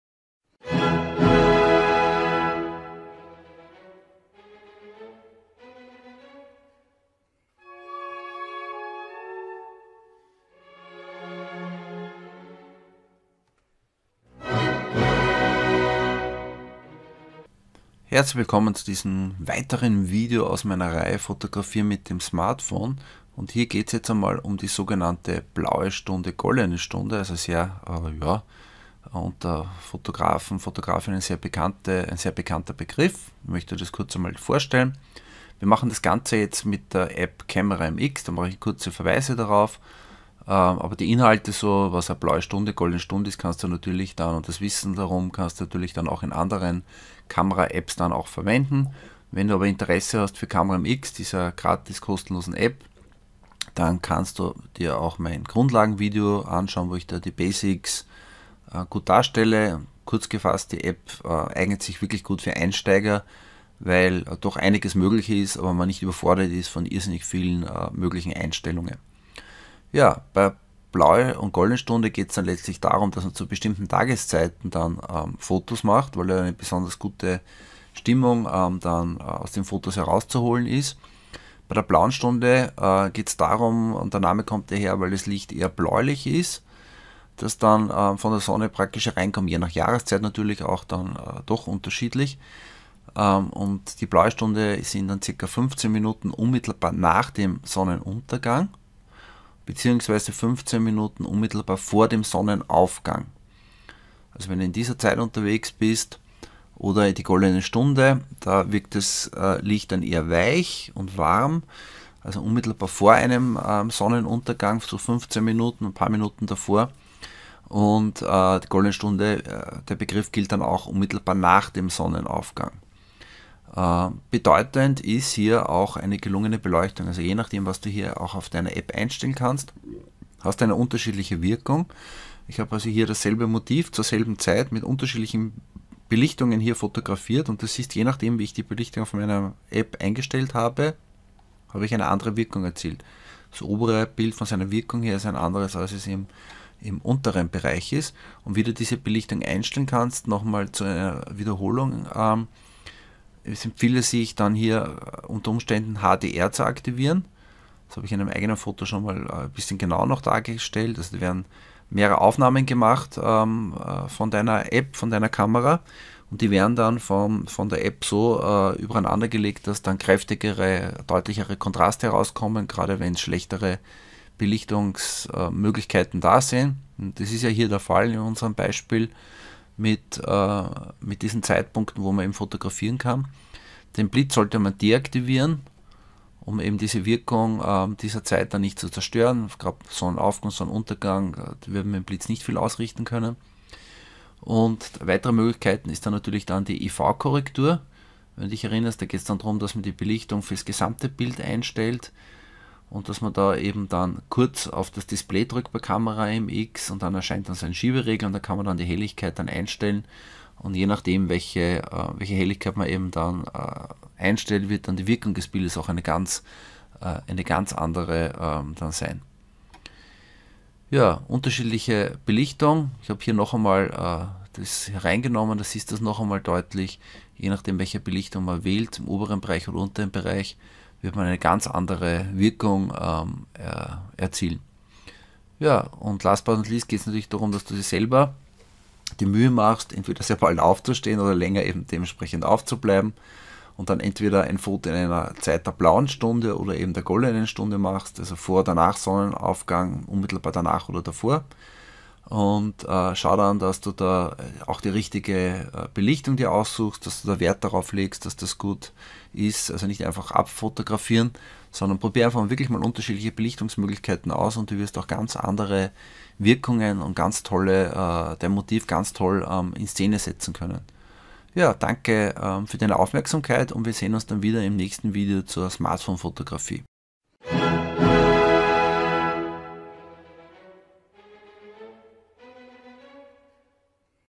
#12 Wie du die "Blaue Stunde" oder "Goldene Stunde" für Smartphone Fotos nützt - App Camera MX
In diesem Video erfährst du wie man mit Hilfe der Wahl des Zeitpunktes der "Blauen Stunde" oder "Goldenen Stunde" gelungene Fotos machen kann. Obwohl ich dies am Beispiel der Android APP Camera MX zeige sind die hier gezeigten Inhalte auch mit anderen Foto - Apps und Smartphones nutzbar. Themen sind u.a. HDR, EV-Korrektur und der Einsatz des Blitzes. Zur Anwendung des Erlernten kannst du eine Übungsaufgabe lösen :-)